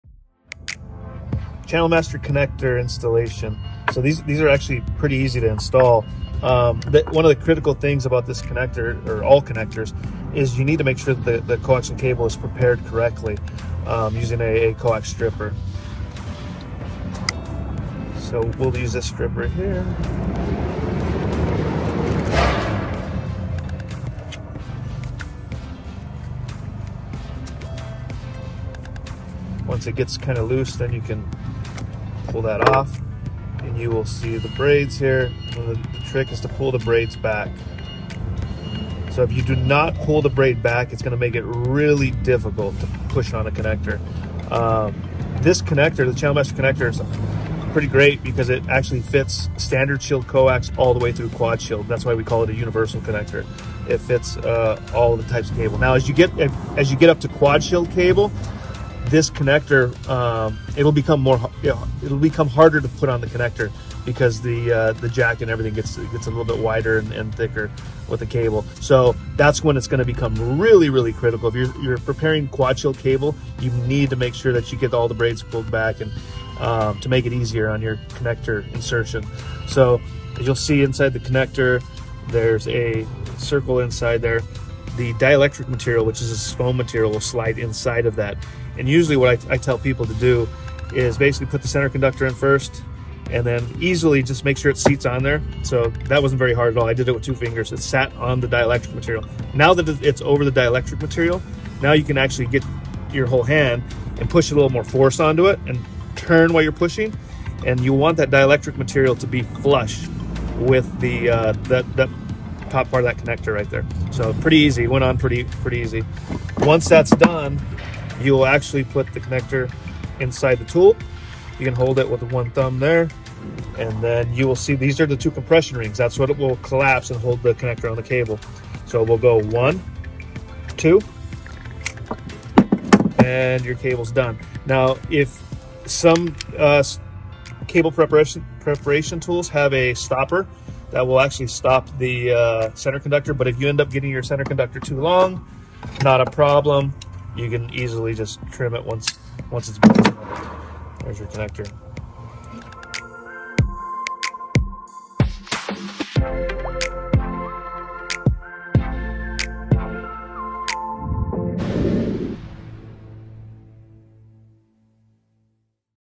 How to Install Compression Connectors on Coaxial Cable | Channel Master
Channel Master Coaxial Compression F-Type Connectors are professional-grade and designed for high-end systems that demand top quality digital audio/video transmissions as well as high-speed data transmissions over coaxial cable.